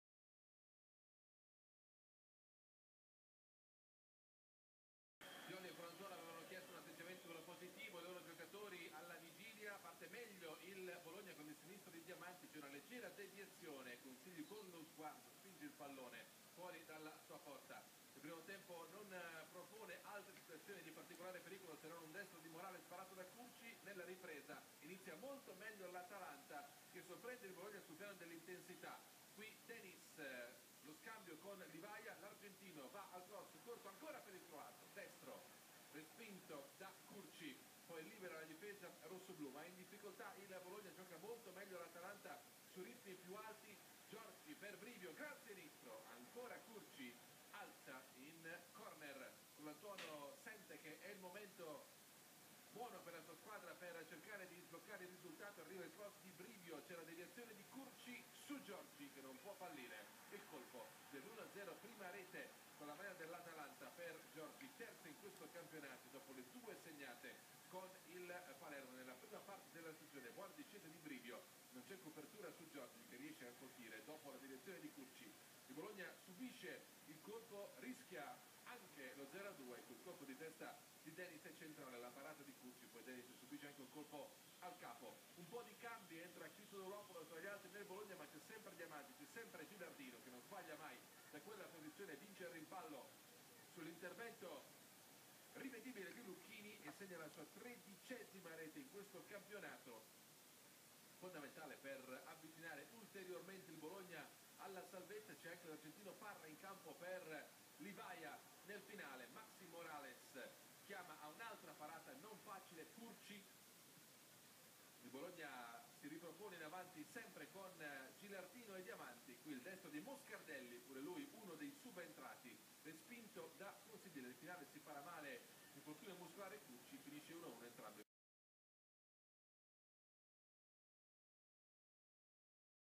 Atalanta 1-1 Bologna - Highlights Sky HD - 27/4/13 - © Serie A 2012-2013
In uno dei due anticipi della 34.ma giornata, Atalanta e Bologna pareggiano 1-1 e si avvicinano ulteriormente alla matematica salvezza. A Bergamo, dopo un primo tempo senza emozioni, i nerazzurri sbloccano la gara con Giorgi (67') ma vengono ripresi poco dopo da Gilardino (76'), bravo a infilare Consigli dopo un rimpallo vinto con Lucchini. La squadra di Colantuono sale a 39 punti mentre gli emiliani raggiungono quota 40.